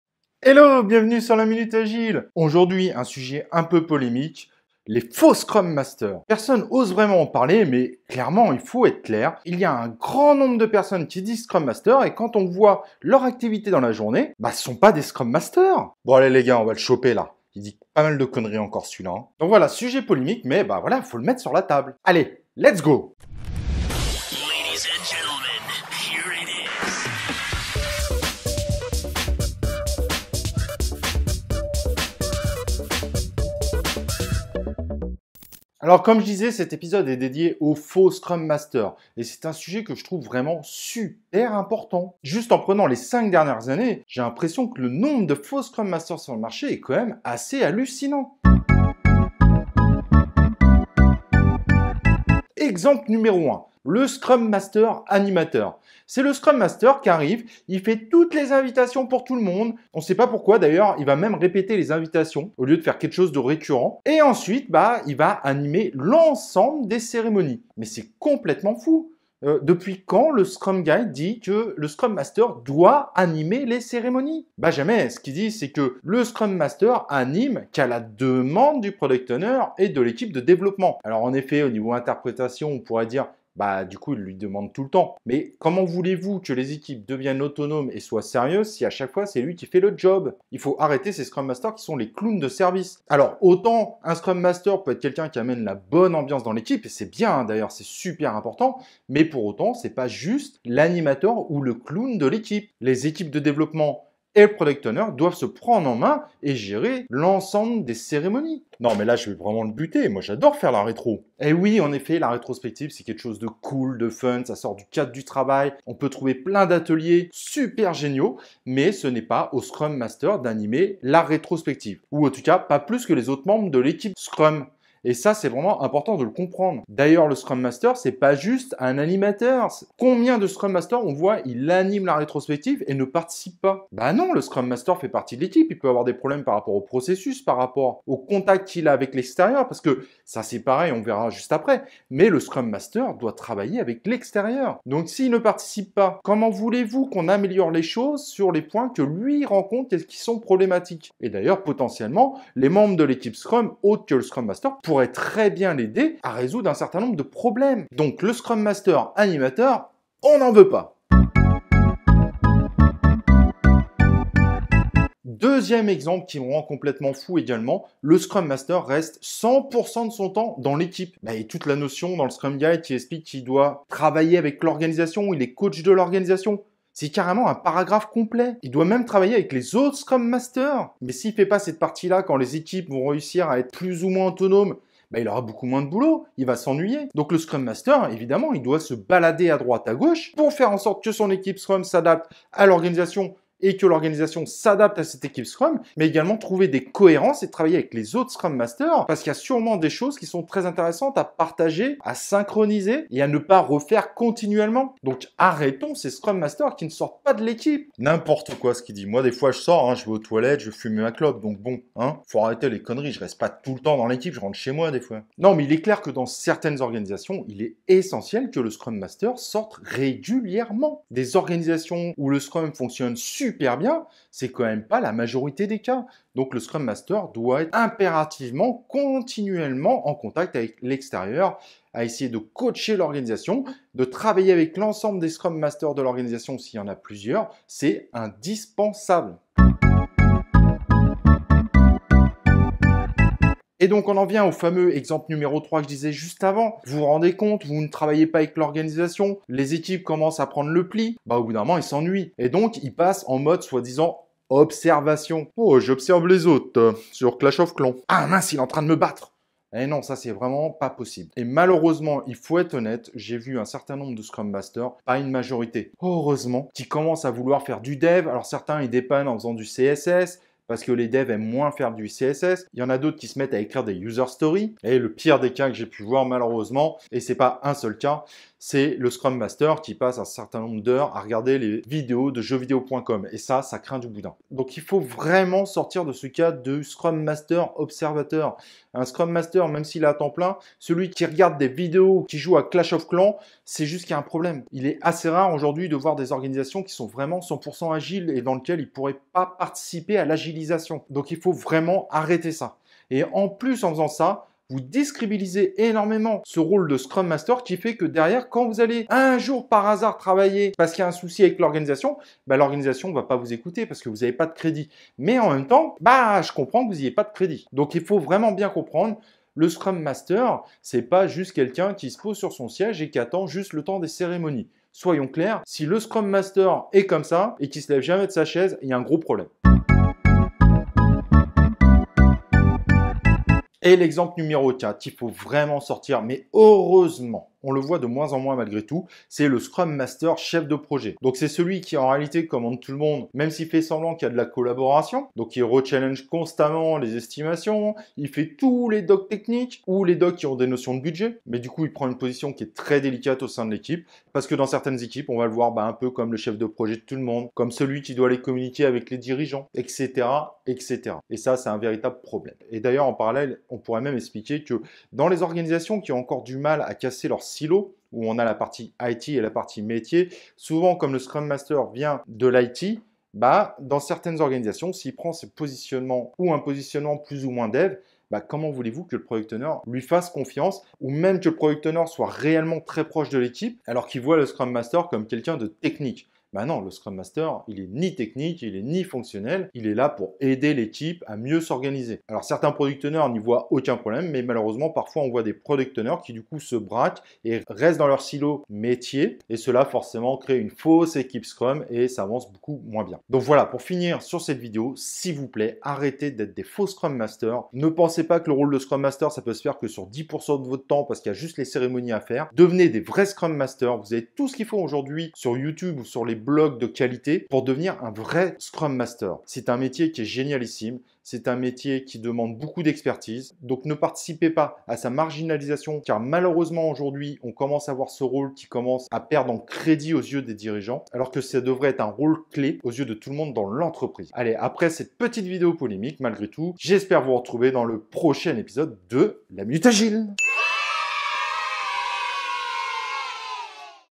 Faux Scrum Master - Scrum Master Agile Scrum
Dénonçons les faux Scrum Master et comprenez e que vous ne devez pas faire en tant que Scrum Master ! Ce rôle se marginalise alors qu'il devrait être clé dans les organisation.

N'oubliez pas de cliquer sur la cloche ("notification")
o Vous recevrez en notification nos nouvelles vidéos
o Très important pour nous soutenir

Auteur et acteur : Paquet Judicaël

(Twitter dédié à La Minute Agile)